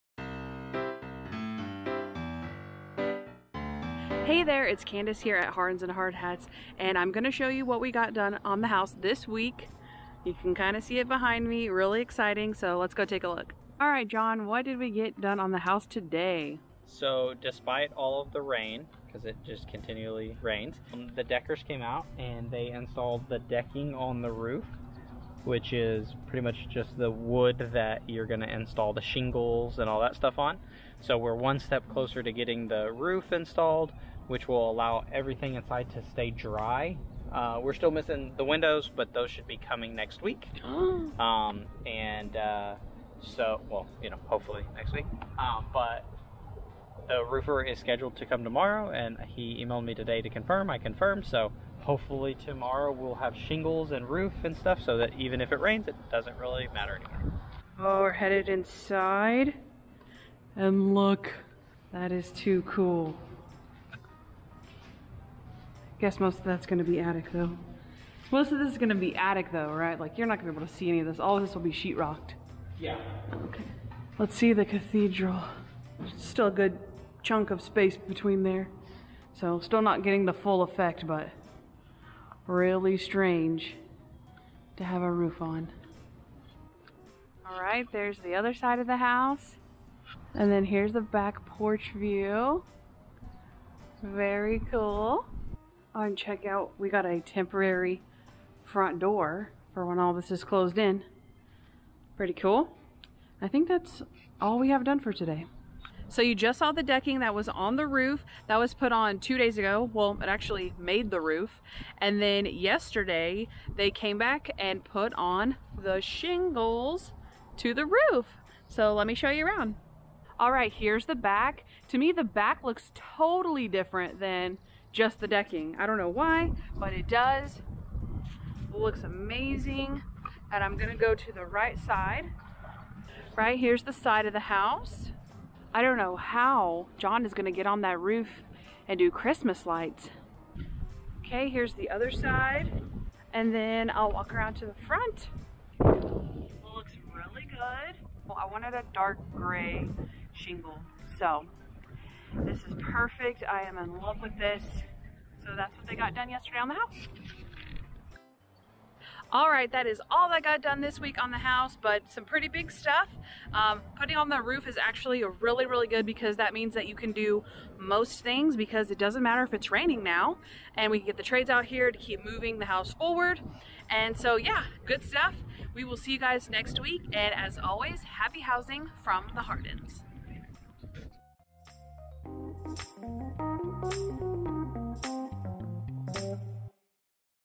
Decking and Shingles on Our Modern Farmhouse Build
This video is all about the weekly progress of our new modern farmhouse located in Mineral Wells, TX. May 23-29, 2021. Follow along with us on our channel as we show every step of our building process!